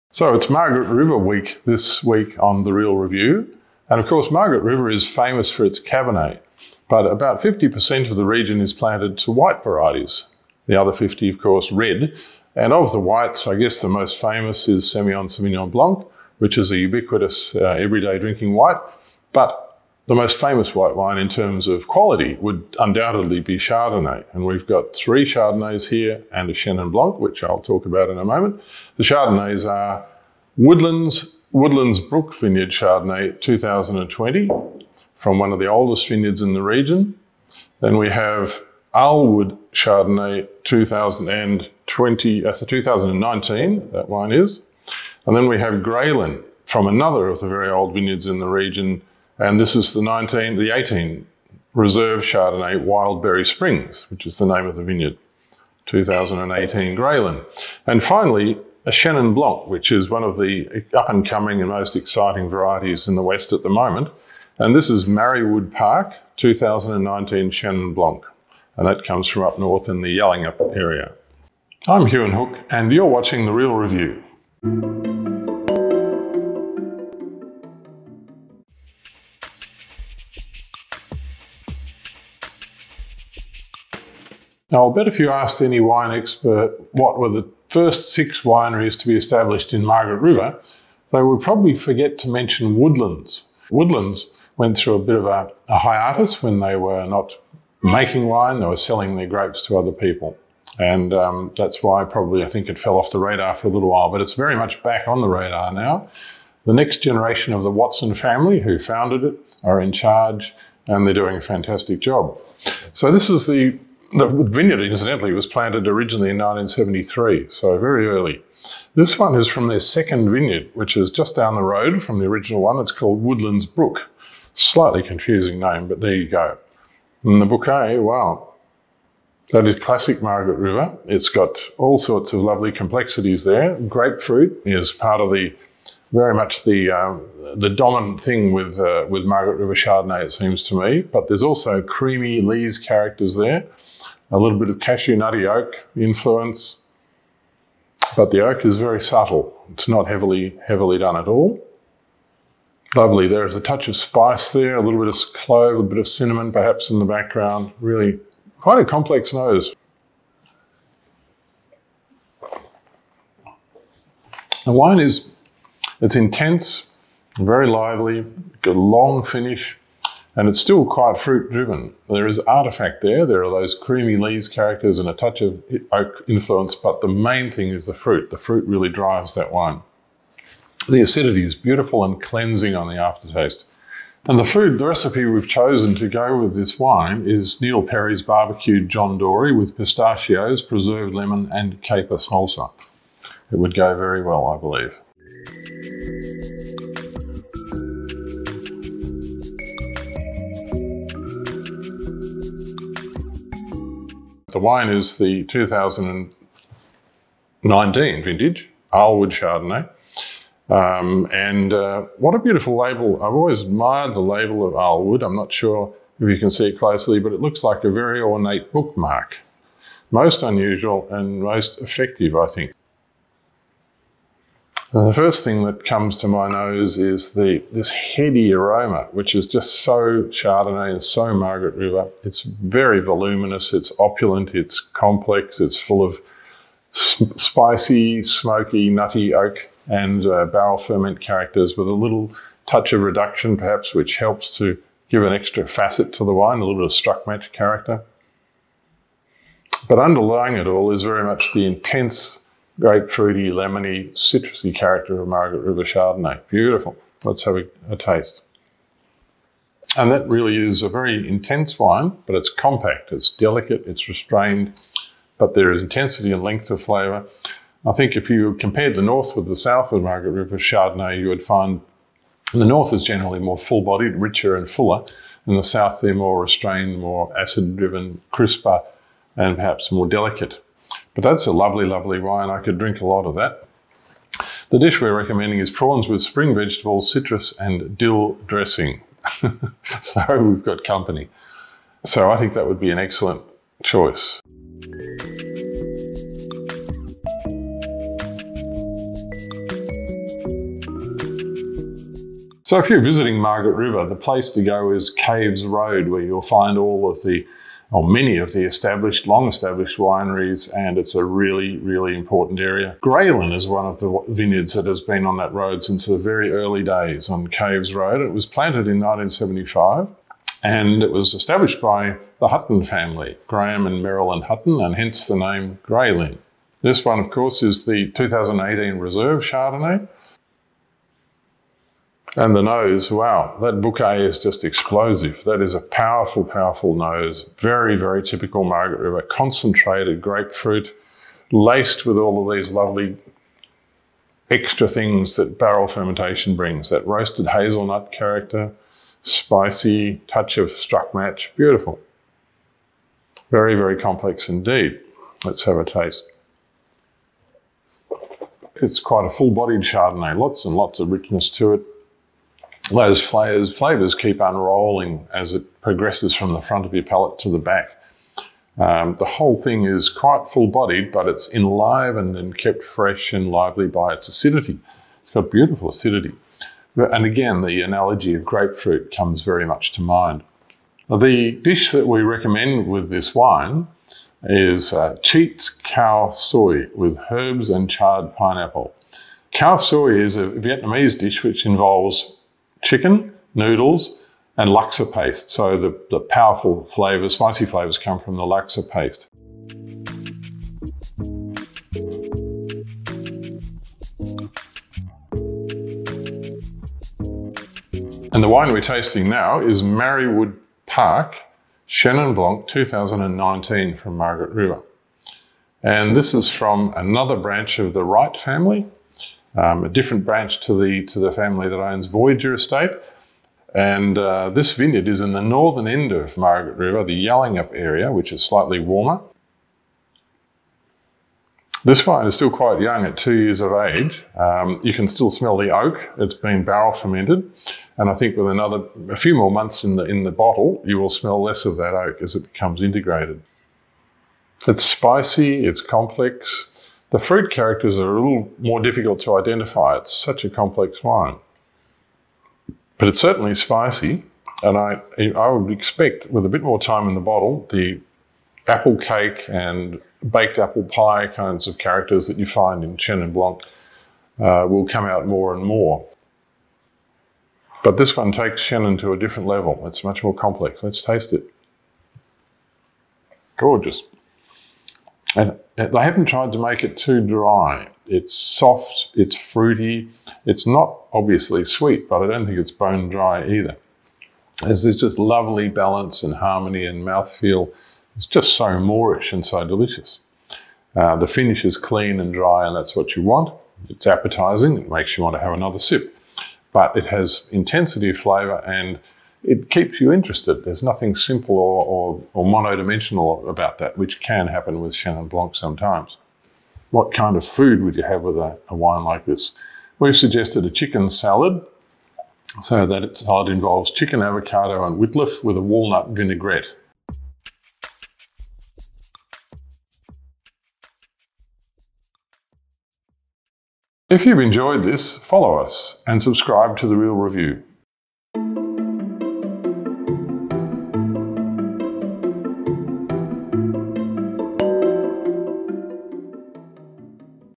Margaret River chenin & chardonnays
It's Margaret River week on The Real Review and of course Margaret River is famous for its cabernet but about 50% of the region is planted to white varieties, the other 50%, of course, red. Of the whites the most famous is semillon sauvignon blanc which is ubiquitous every day drinking white but the most famous white wine in terms of quality would undoubtedly be chardonnay. We've got three chardonnays here and a chenin blanc.

The chardonnays are:

Woodlands Woodlands Brook Vineyard Chardonnay 2020

Arlewood Estate Chardonnay 2019

Gralyn Estate Reserve Chardonnay 2018

The chenin blanc is:

Marri Wood Park Chenin Blanc 2019